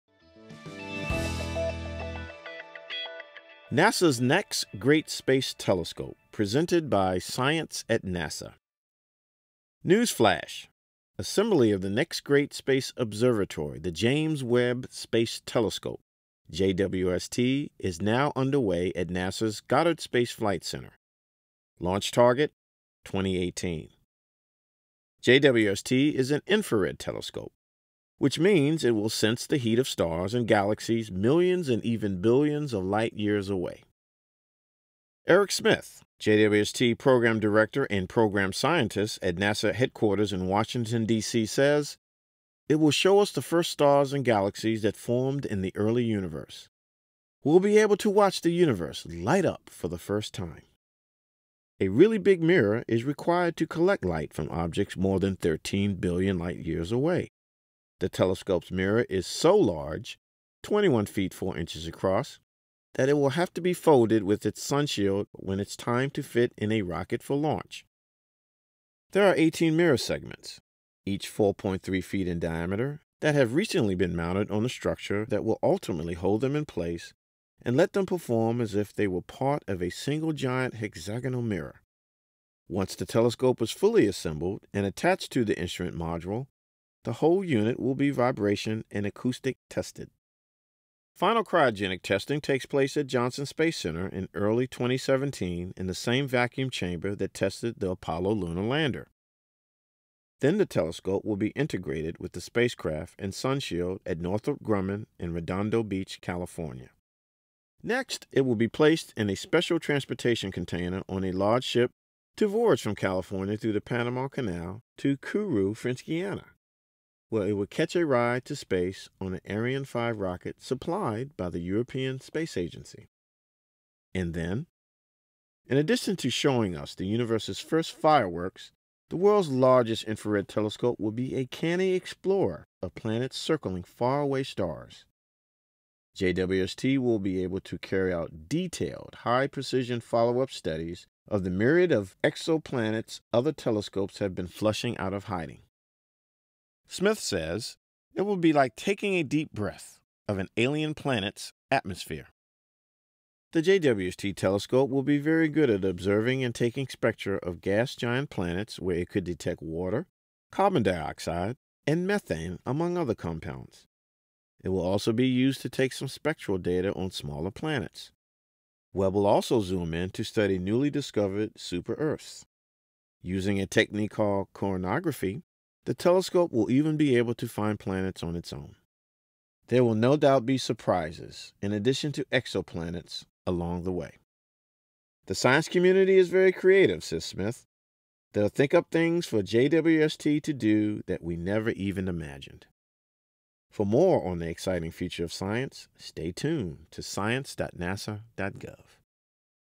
ScienceCasts: NASA's Next Great Space Telescope
Assembly of NASA's next great space observatory, the James Webb Space Telescope, is underway. It will show researchers the first stars and galaxies that formed in the early universe.